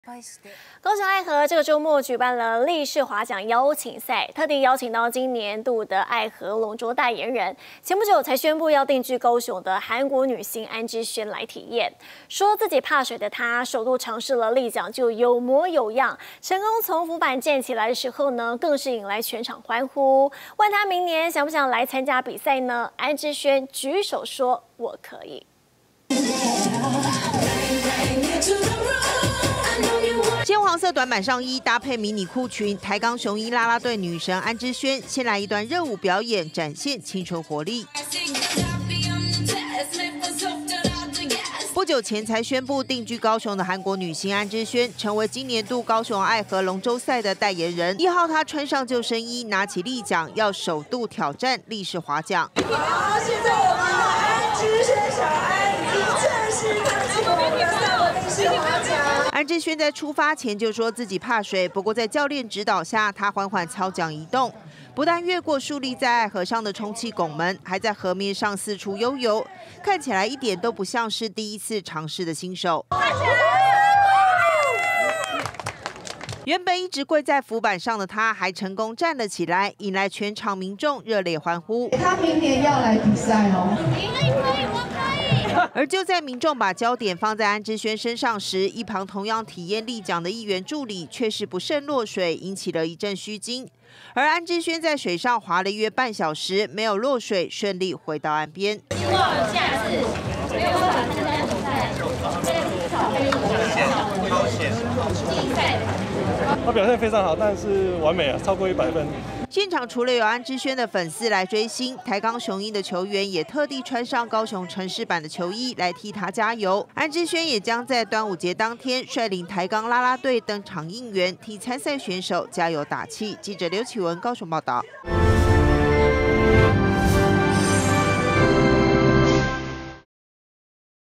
女神來了！ 首度挑戰立槳賽 安芝儇架式十足
高雄愛河這個周末舉辦立式划槳邀請賽，特地邀請今年度愛河龍舟代言人，前不久才宣佈定居高雄的韓國女星安芝儇來體驗，說自己怕水的安芝儇，首度嘗試立槳就有模有樣，成功從浮板上站起來時，更是引來全場歡呼，問她明年想不想來參加比賽，安芝儇還連忙舉手說我可以。